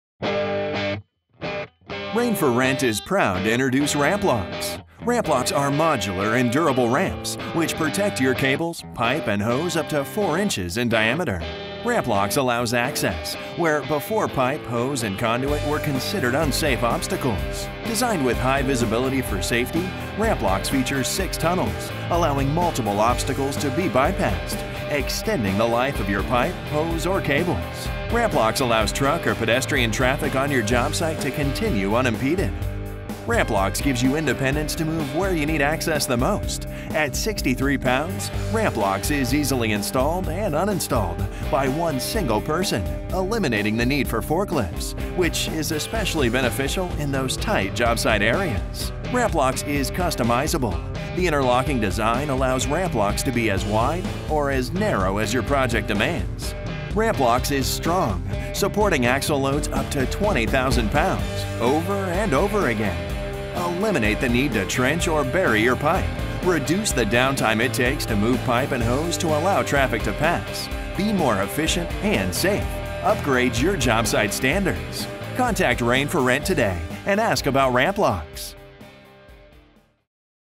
RampLox® Modular Pipe and Hose Ramp System by Rain for Rent
Rain for Rent's unique RampLox® U.S. Patent # D725,748 is a modular pipe and hose ramp system that protects your hose, pipe, and conduit and allows vehicle or pedestrian traffic to continue unimpeded. Info